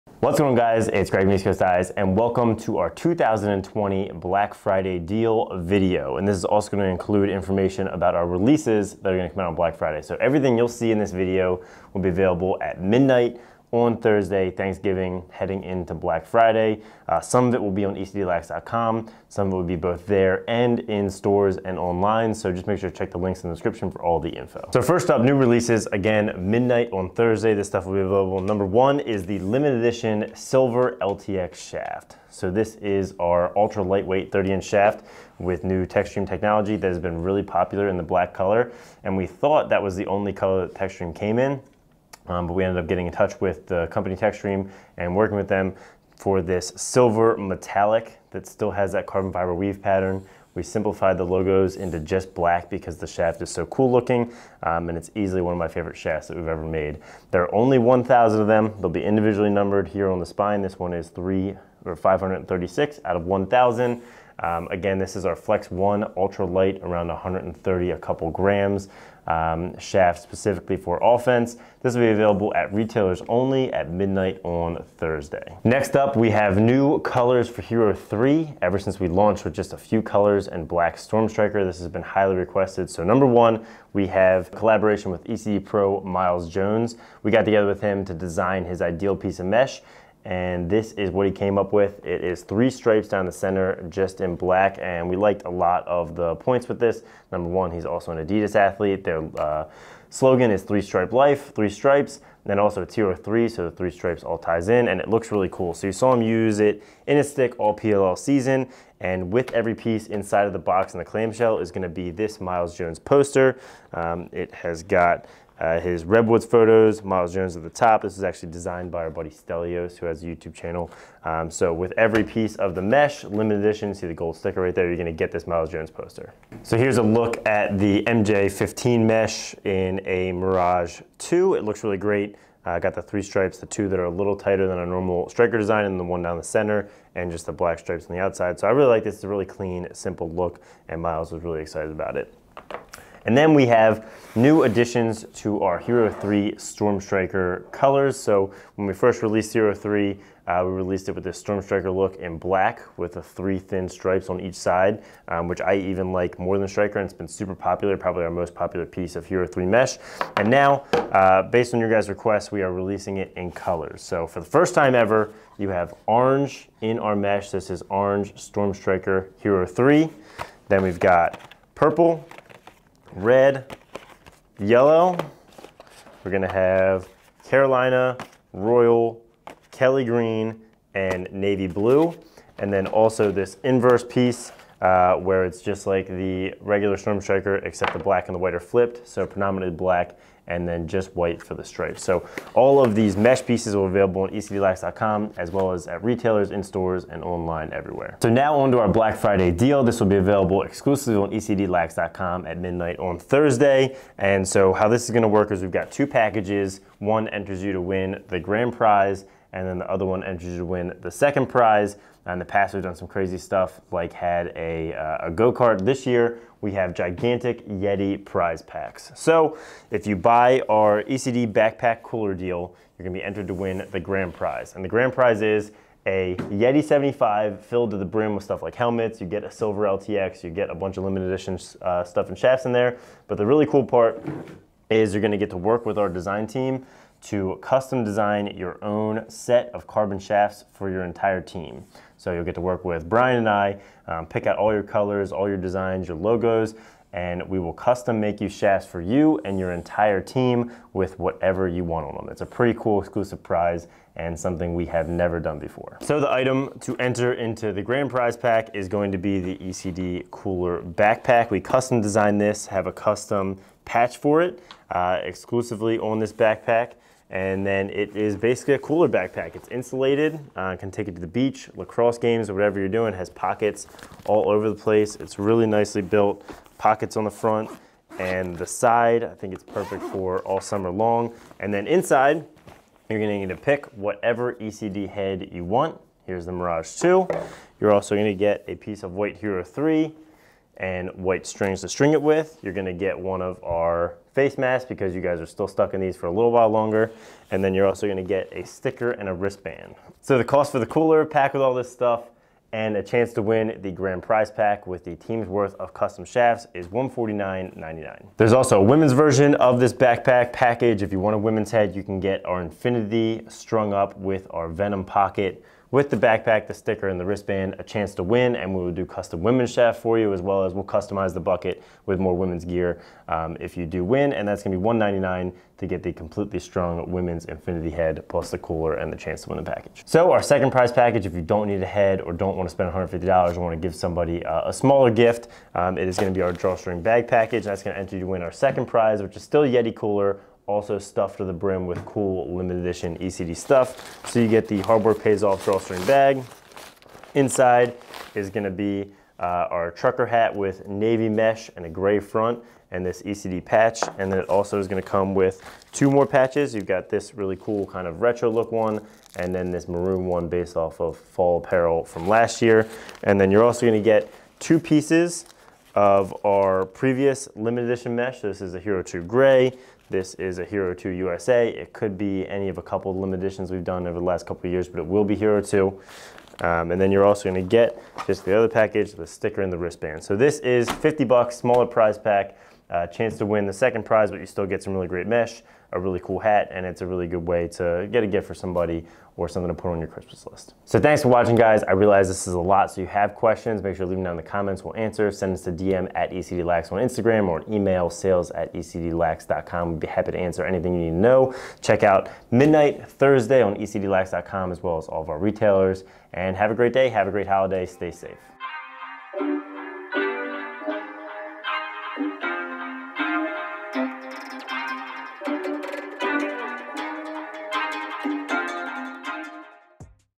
ECD Black Friday 2020 + Hero 3.0 Colors + LE Silver LTX
Black Friday 2020 will be like no other. 2 releases as well as a huge give away! ECD Greg gives you all the details about our new Hero 3.0 colors, LE Silver LTX, and how to enter our Black Friday Contest!!